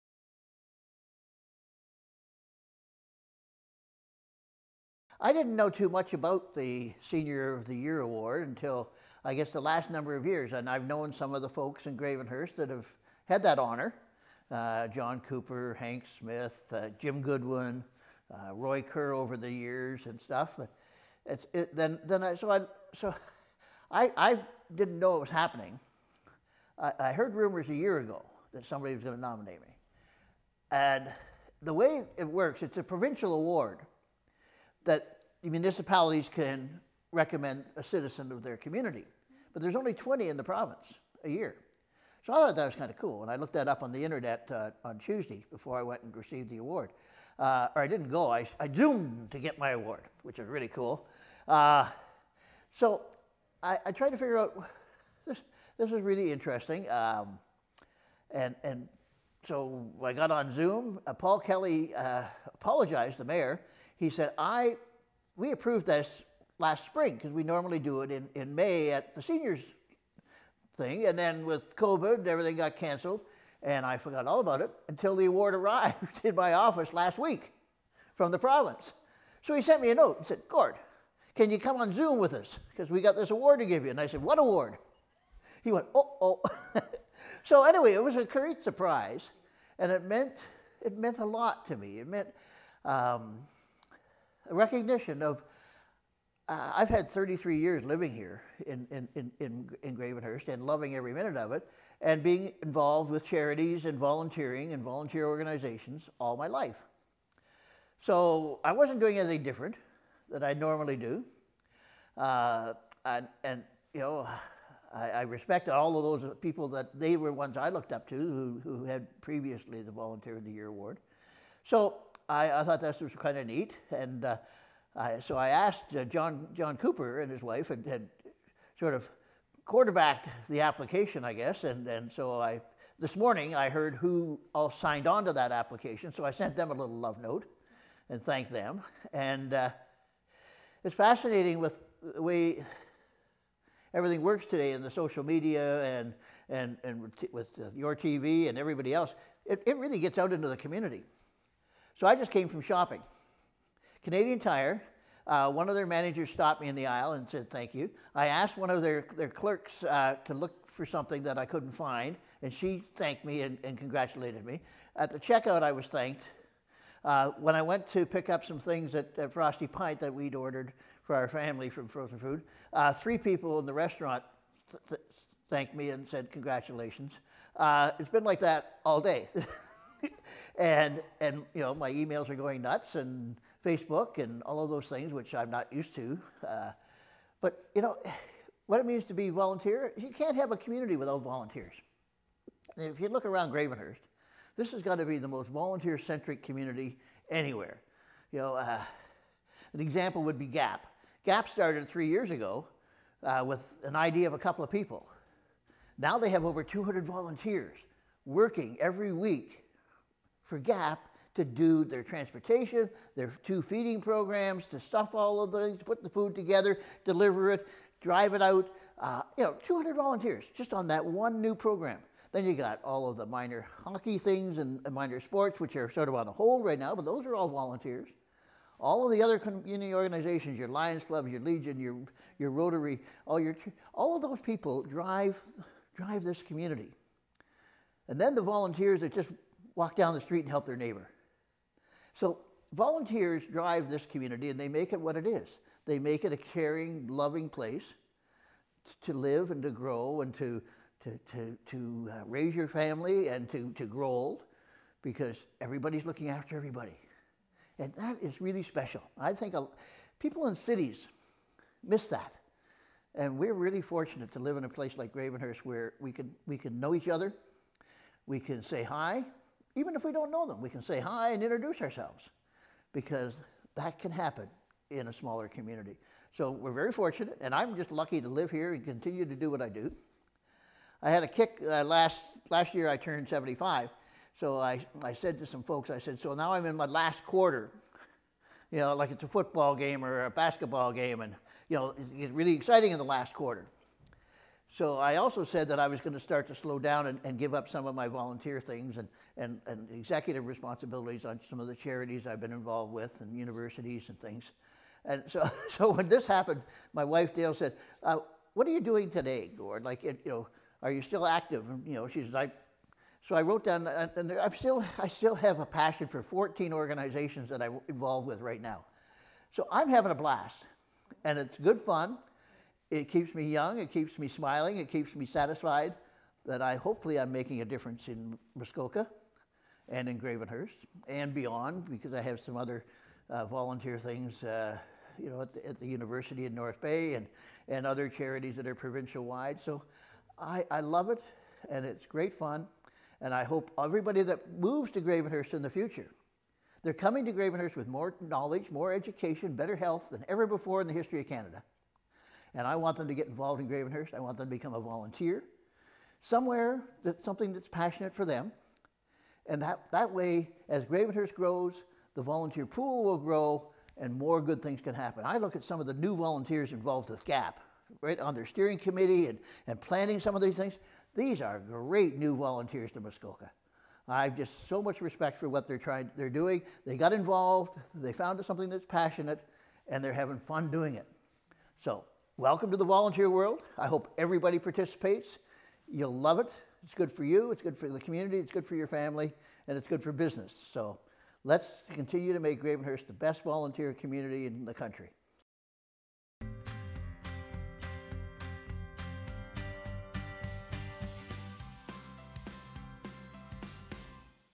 Senior of the Year Gord Durnan reflects on his award
Gord Durnan talks about his nomination as Gravenhurst's Senior of the Year.